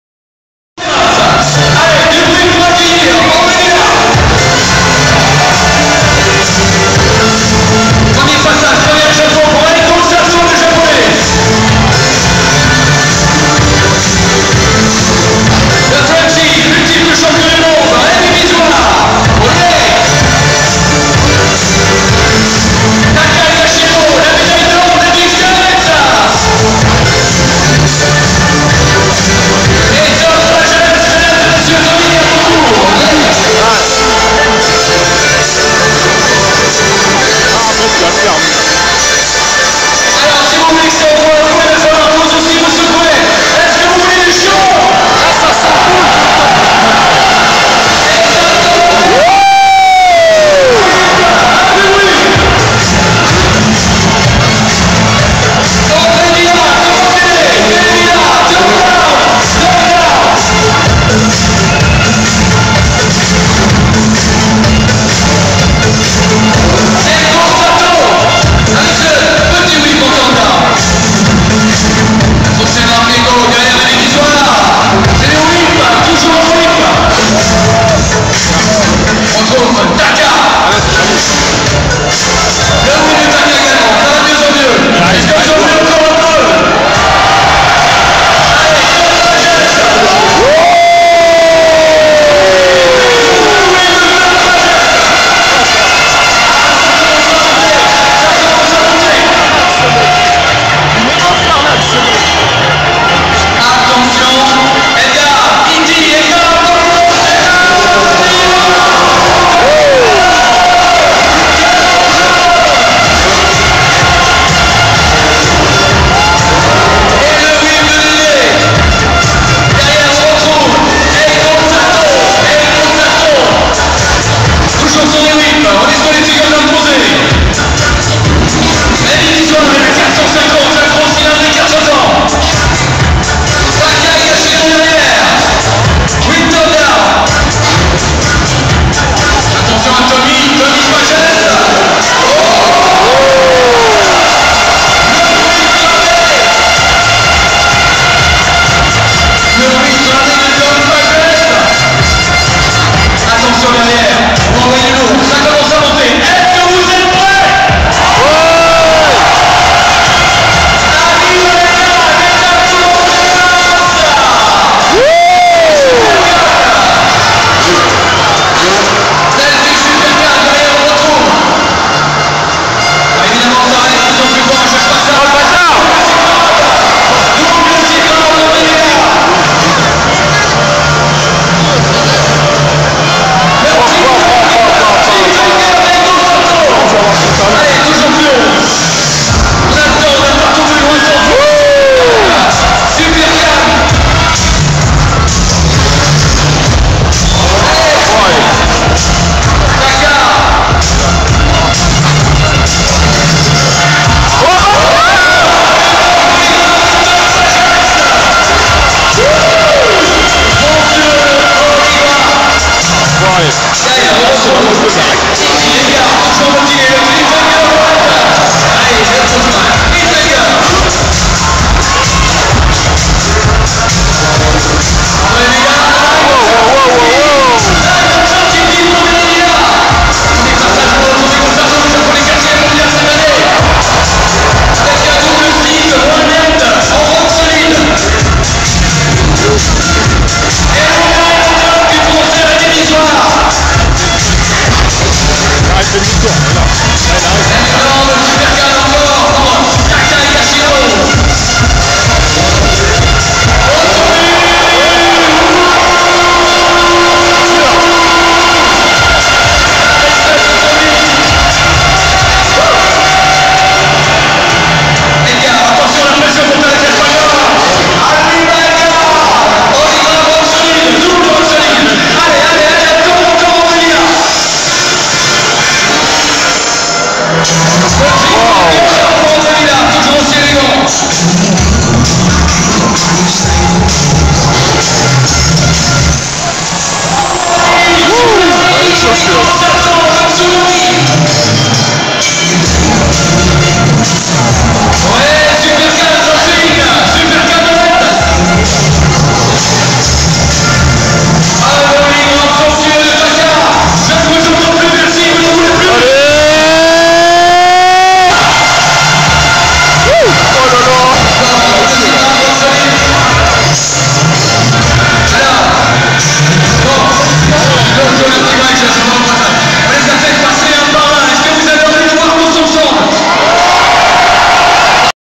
Supercross paris becy 2012 freestyle
Sequence freestyle avec des sauts d'anthologie!